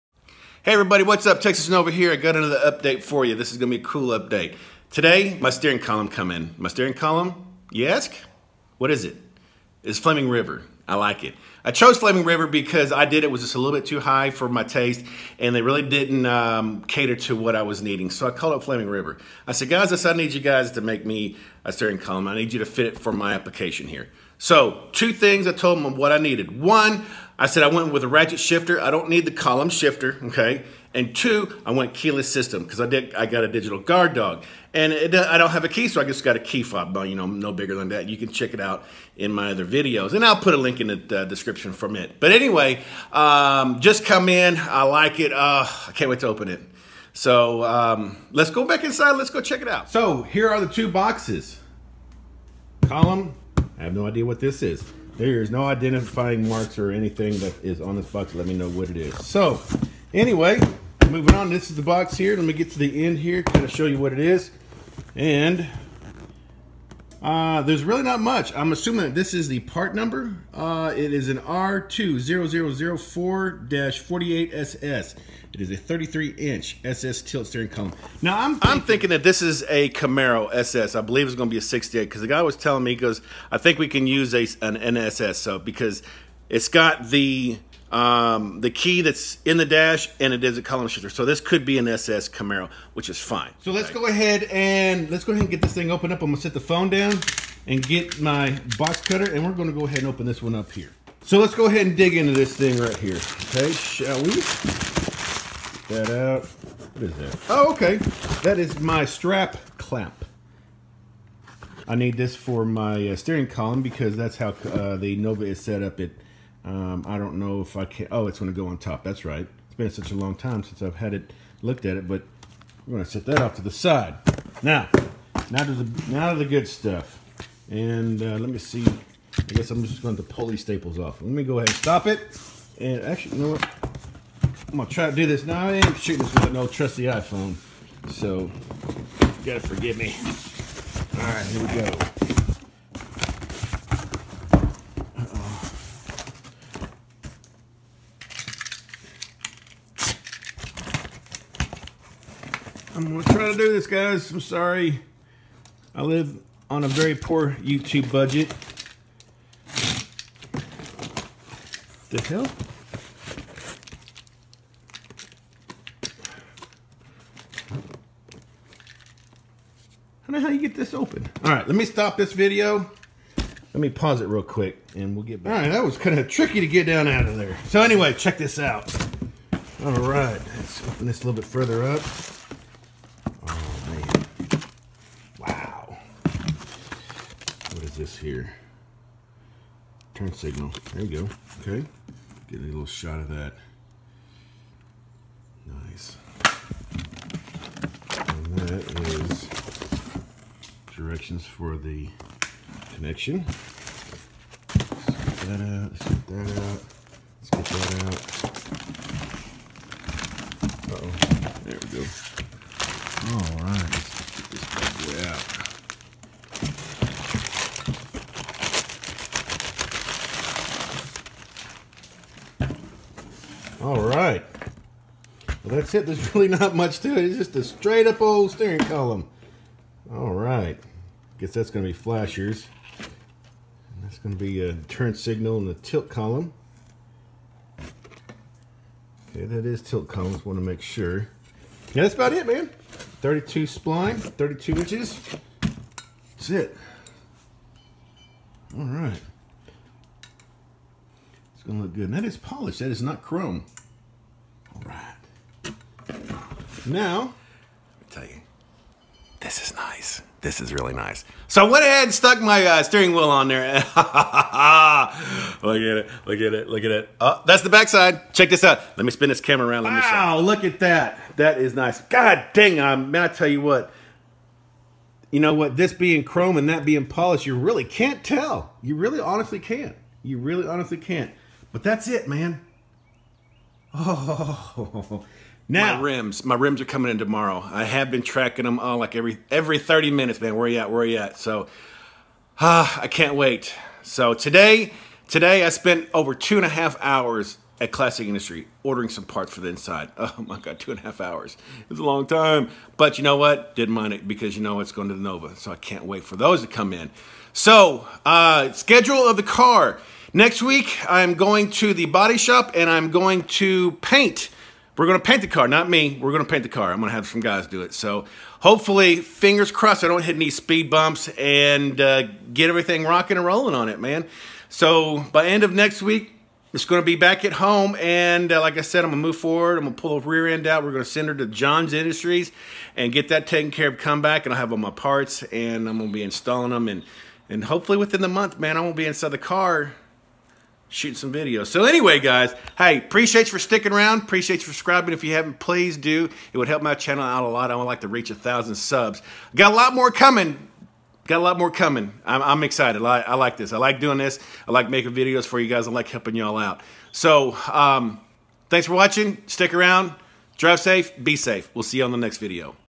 Flaming River is on FIRE!!!!
My Flaming River Column Came in in Today & man it is HOT!
***Correction - The Column is 33 inches***